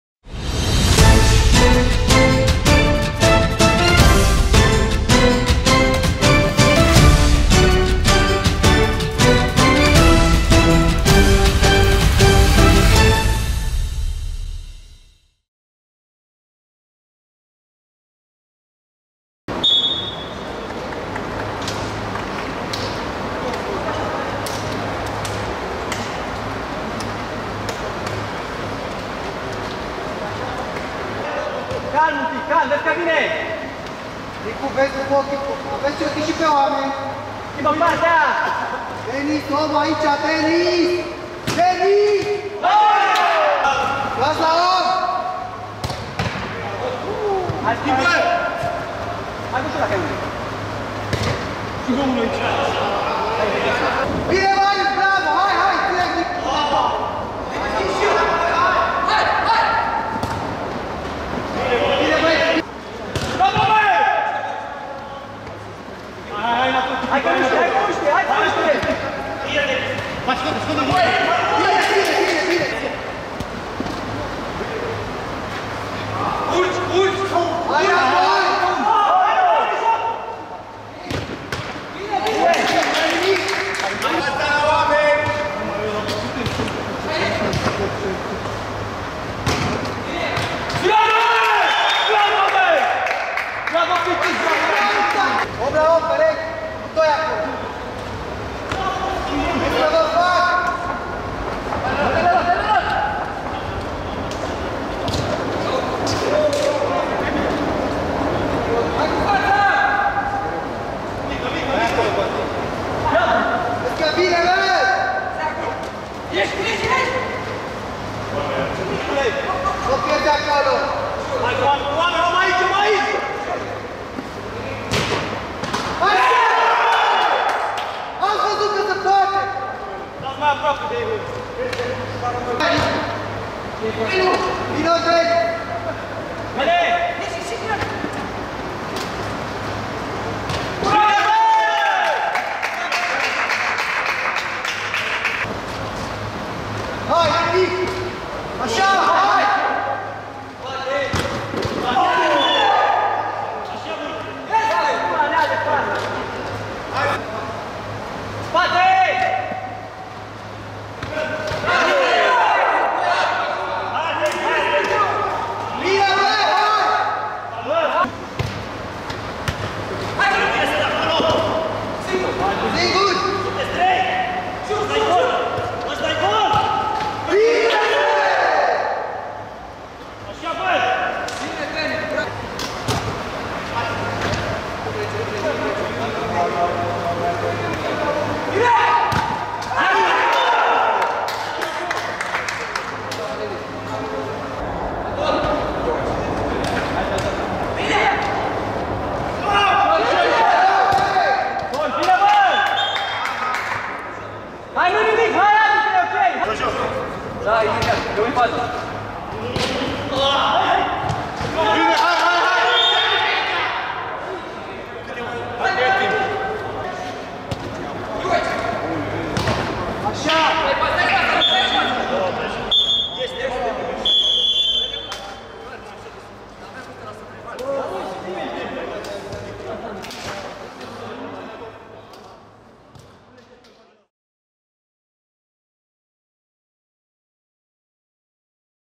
Rezumat Ericsson vs Deloitte (1.Liga Companiilor 2019/20)
• 18/12/2019 | 2.Liga Companiilor 2019/20 | Ediția 11 | Highlights - meci et.10.
• partener AFAN | sponsori Lotto si Cumpana | membru România în European Football Business Leagues United.